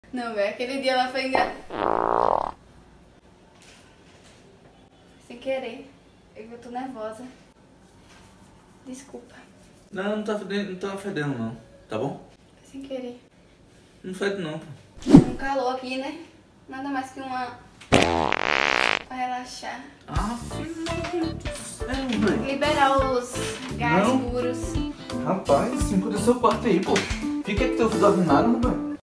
Peidando perto do boy pela primeira vez
O primeiro peido perto do namorado a gente nunca esquece,mas depois acostuma kkkk